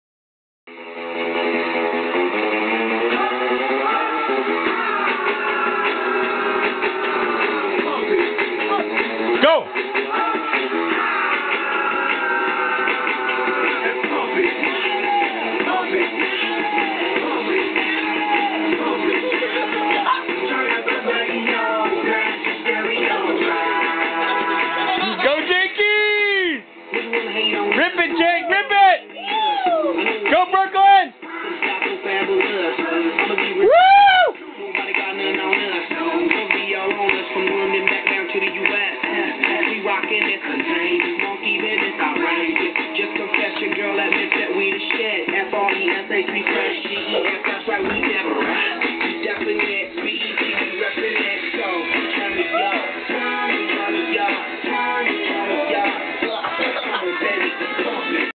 Jake Pasmore does the The snorkle dance while Nick & Brooke Watch
Jake Pasmore does the The snorkle dance